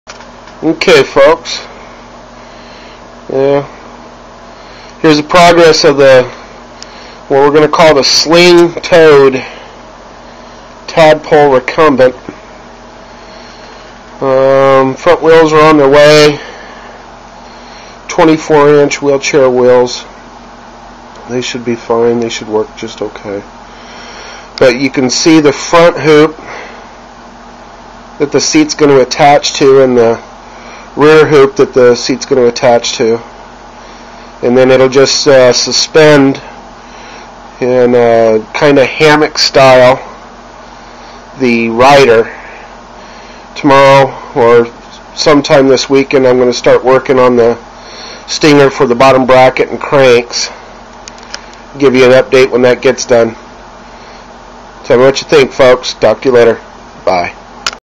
SlingToad.MOV
Here is my progress of my tadpole recumbent...